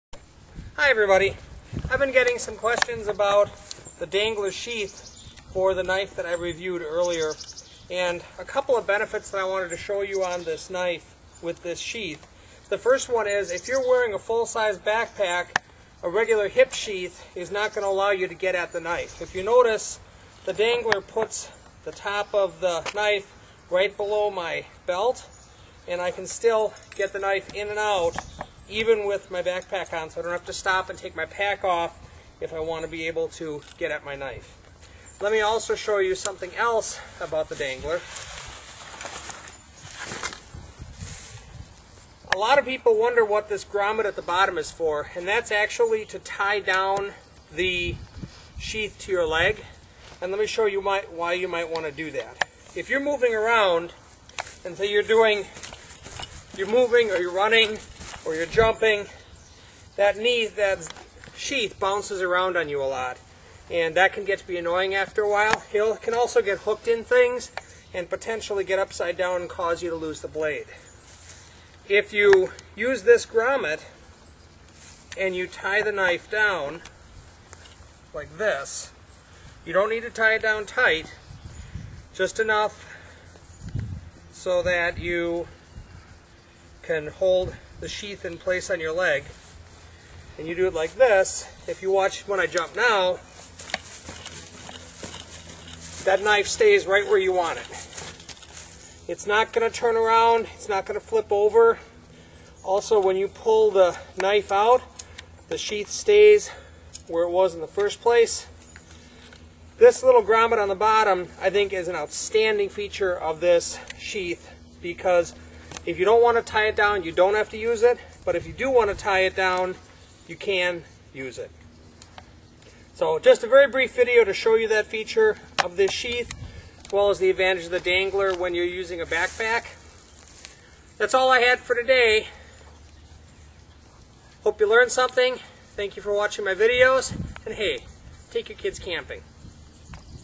Dangler Sheath Benefits
A review of the benefits of the dangler sheath VS the hip sheath the the Aniolek knife from Blind Horse knives.